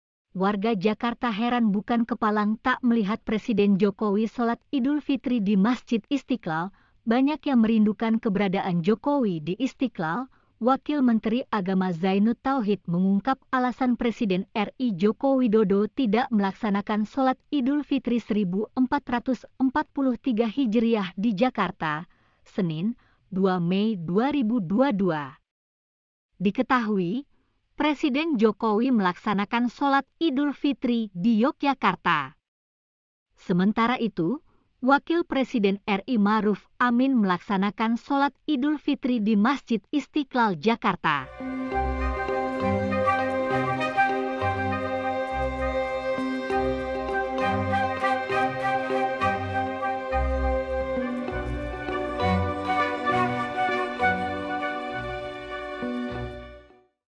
🔴 Ini Alasan Jokowi Shalat Idul Fitri di Yogyakarta, bukan di Masjid Istiqlal Jakarta
Warga Jakarta heran bukan kepalang tak melihat Presiden Jokowi sholat Idul Fitri di Masjid Istiqlal.

Banyak yang merindukan keberadaan Jokowi di Istiqlal.

Wakil Menteri Agama Zainut Tauhid mengungkap alasan Presiden RI Joko Widodo tidak melaksanakan Shalat Idul Fitri 1443 Hijriah di Jakarta, Senin (2/5/2022).

Diketahui, Presiden Jokowi melaksanakan Shalat Idul Fitri di Yogyakarta. Sementara itu, Wakil Presiden RI Ma'ruf Amin melaksanakan Shalat Idul Fitri di Masjid Istiqlal Jakarta.